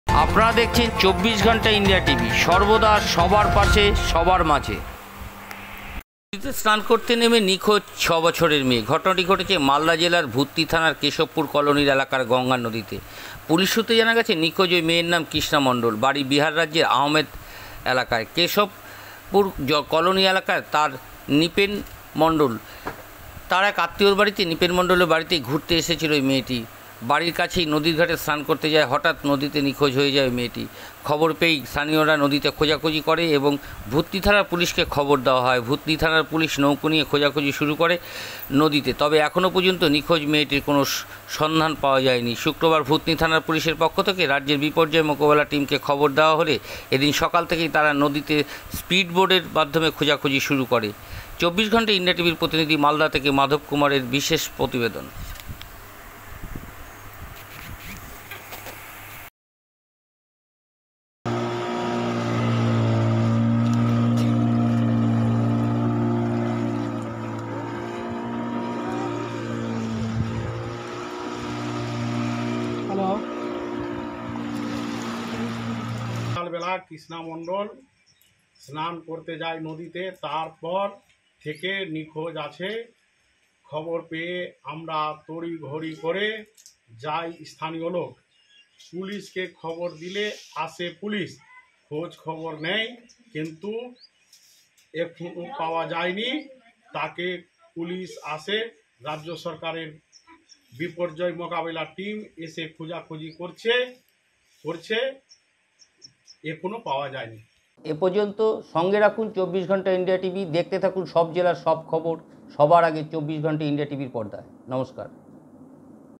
নদীতে শান করতে নেমে নিখোঁজ এক 6 বছরের মেয়ে
নদীতে শান করতে নেমে নিখোঁজ এক 6 বছরের মেয়ে। ঘটনাটি ঘটেছে মালদা জেলার ভুতনি থানার কেশবপুর কলোনি এলাকায় গঙ্গা নদীতে। পুলিশ সূত্রে জানা গেছে নিখোঁজ ওই মেয়ের নাম কৃষ্ণা মণ্ডল বাড়ি বিহার রাজ্যের আমদাবাদ এলাকায়। কেশবপুর কলোনী এলাকার তার নিপেন মন্ডলের
এক আত্মীয়র বাড়ি নিপেন মন্ডলে বাড়িতে ঘুরতে এসেছিল মেয়েটি। বাড়ির কাছের নদী ঘাটে সান করতে যায় তারপরে হঠাৎ নদীতে নিখোঁজ হয়ে যায়। ঘটনার খবর পেয়ে স্থানীয়রা নদীতে খুঁজাখুঁজি করে ও ভুতনি থানার পুলিশকে খবর দেওয়া হয়। ভূতনি থানার পুলিশ নৌকো নিয়ে খোঁজাখুঁজি শুরু করে নদীতে তবে এখন পর্যন্ত নিখোঁজ মেয়েটির কোন সন্ধান পাওয়া যায়নি। শুক্রবার ভুতনি থানার পুলিশের পক্ষ থেকে রাজ্য বিপর্যয় মোকাবিলায় টিমকে খবর দেওয়া হলে এদিন সকাল থেকে তারাও নদীতে স্পিড বোটের দিয়ে খোঁজাখুঁজি শুরু করে। তবে শুক্রবার দুপুর পর্যন্ত নিখোঁজ মেয়ের এখনো সন্ধান কোন রকম পাওয়া যায়নি।